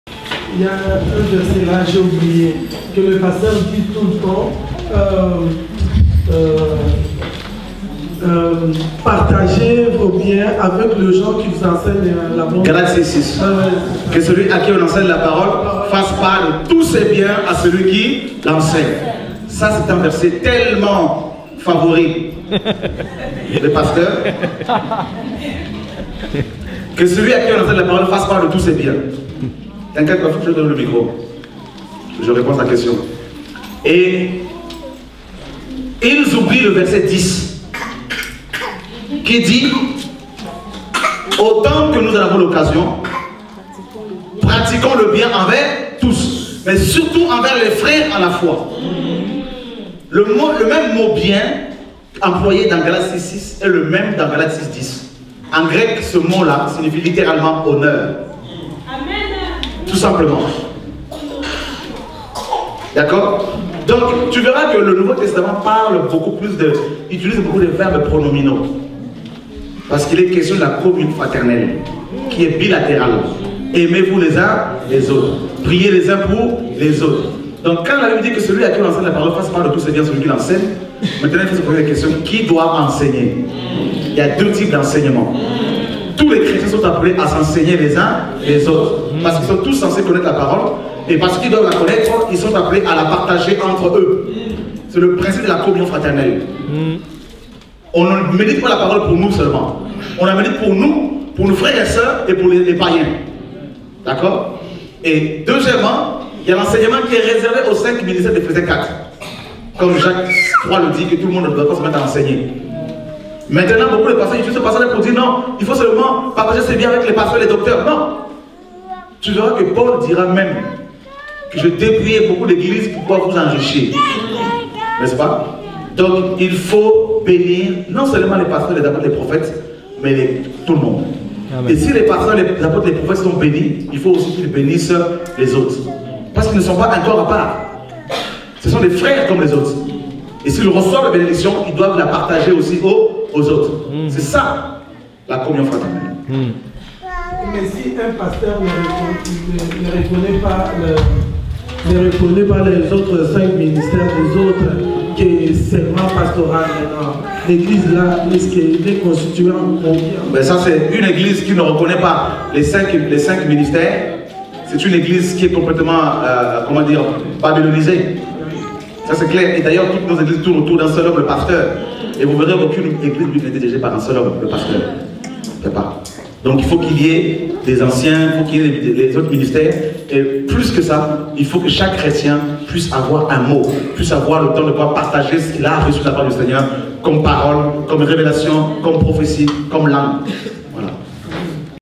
Galates 6 v 6 - La Communion fraternelle - L'Enseignement - (Shora Kuetu)
Galates 6:6 --  Que celui à qui l'on enseigne la parole fasse part de tous ses biens à celui qui l'enseigne.

Galates 6:10  --  Ainsi donc, pendant que nous en avons l'occasion, pratiquons le bien envers tous, et surtout envers les frères en la foi.